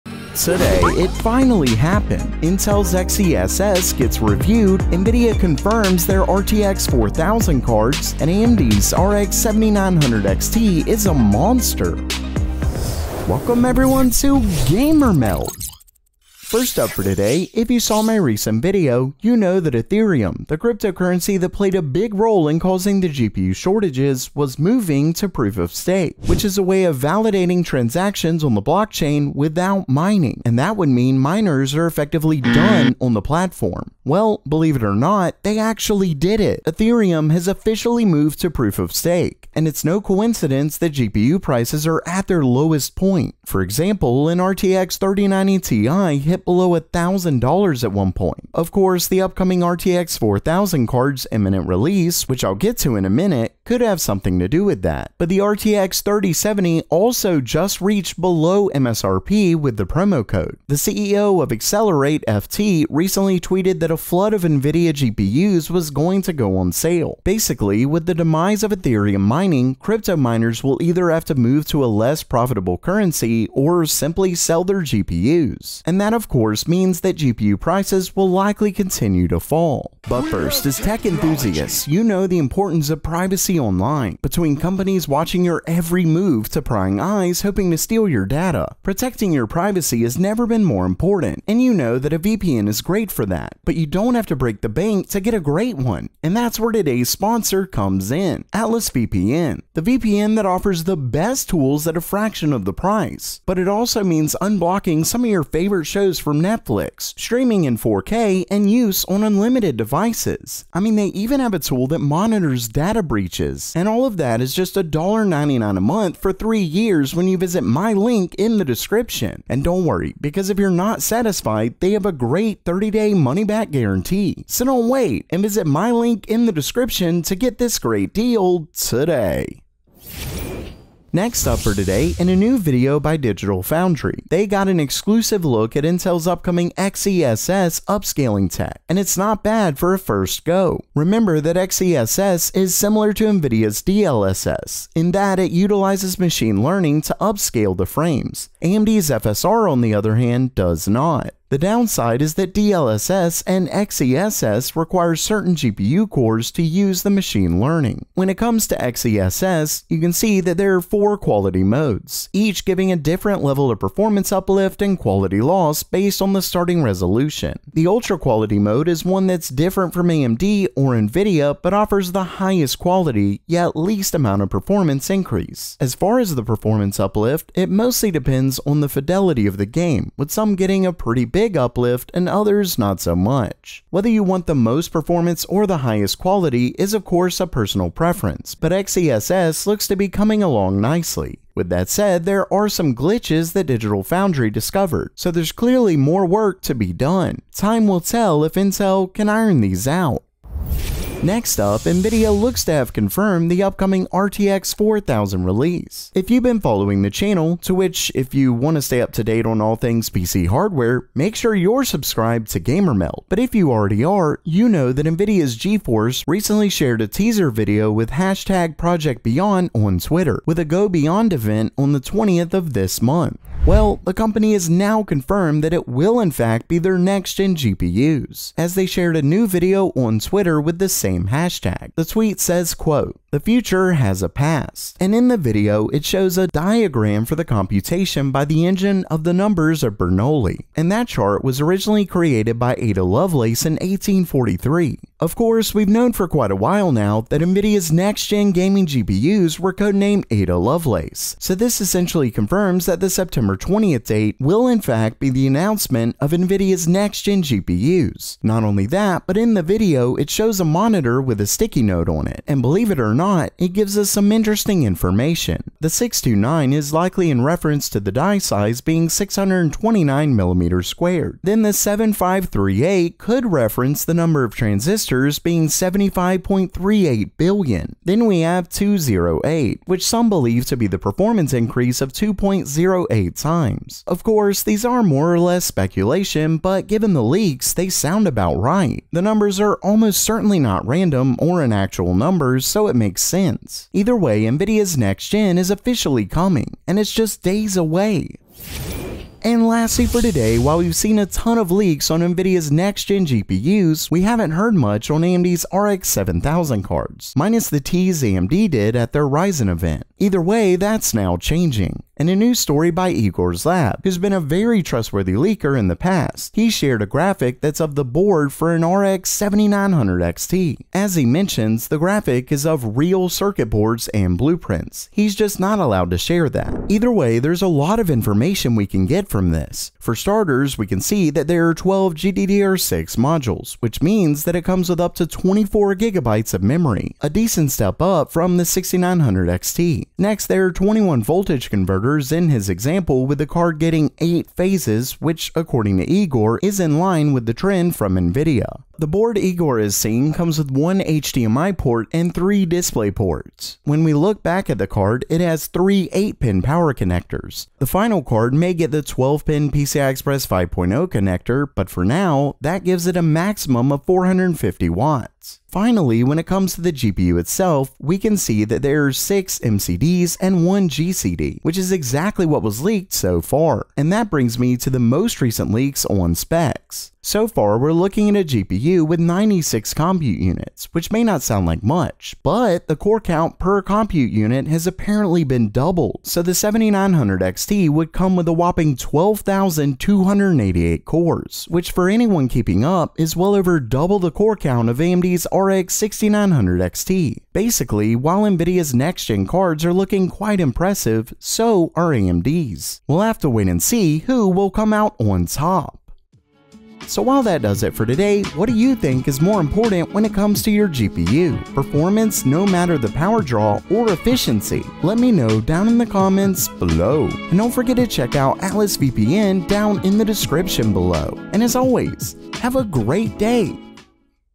RX 7900 XT CHANGES GPUs Forever!
►Grab Atlas VPN for just $1.99 a month for a 3-year subscription

AMD's RX 7900 XT is amazing, Nvidia confirms their RTX 4000 GPUs, Intel's XESS gets a review and the day has finally come!

TIMESTAMPS
0:00 Intro
0:17 It Finally Happened
2:10 Intel XESS Review
3:20 Nvidia Confirms Their RTX 4000 GPUs
4:59 RX 7900 XT Is A Monster